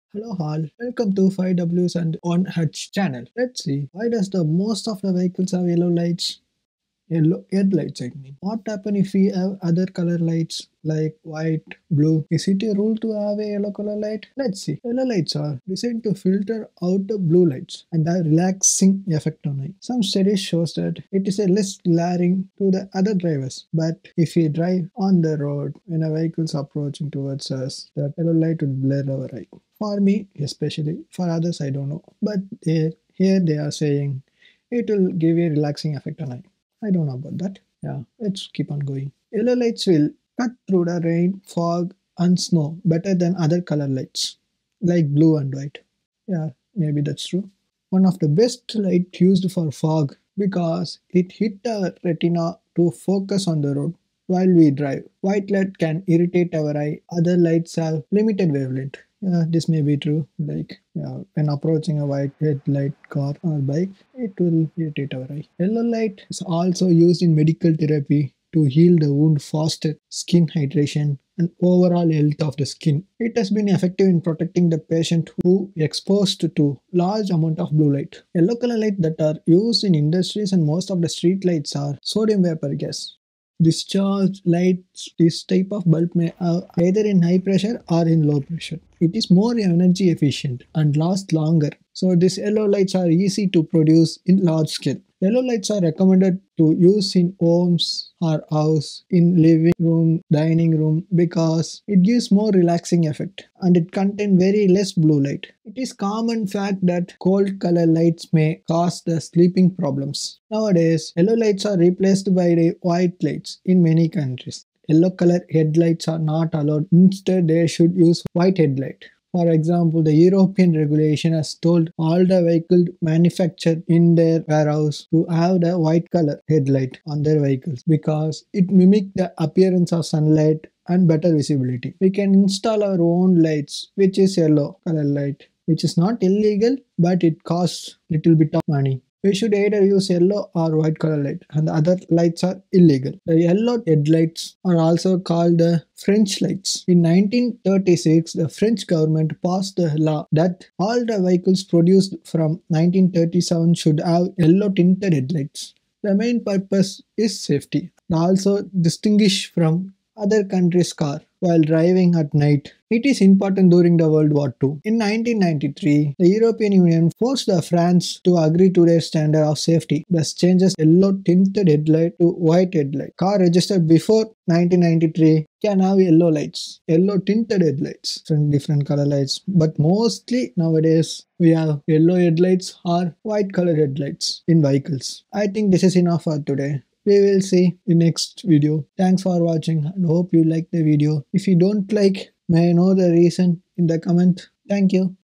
The Science Behind Yellow Vehicle Headlights | Unraveling the Mystery
Welcome to Queriosity Quest! In this captivating episode, we delve into the fascinating world of vehicle headlights and explore the reasons why some are yellow. Have you ever wondered why certain cars emit a warm, golden glow on the road instead of the traditional white light? Join us on this enlightening journey as we uncover the science behind yellow headlights and their surprising advantages.

Why do most of the vehicles have yellow lights?
What happens if we have other color lights?
Why yellow lights?

Yellow lights are one of the most used on both streets and vehicles. It has many uses it is first used in french cars to differentiate the car from others during world war - II.

Other Videos if you want to watch...